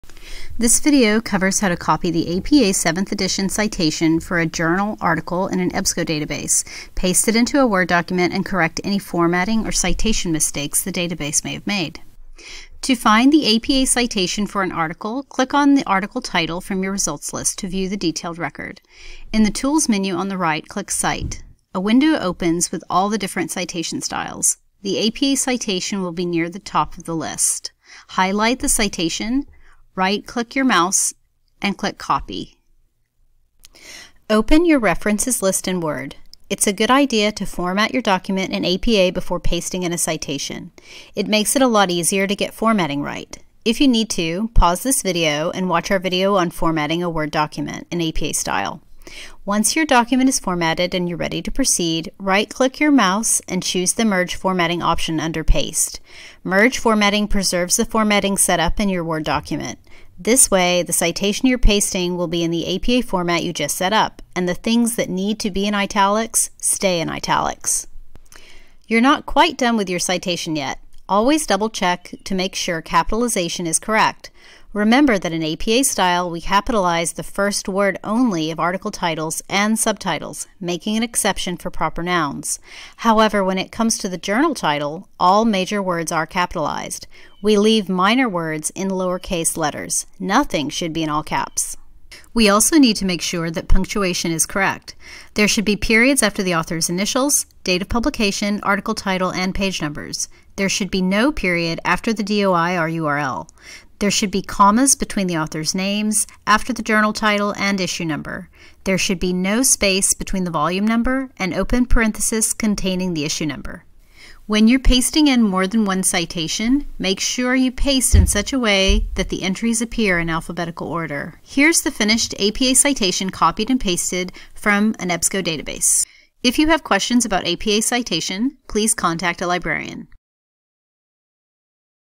Cite a Journal Article from EBSCO in APA 7th ed.
This video describes how to copy an APA citation for a journal article in an EBSCO database and paste it in Word, making sure it stays in APA format.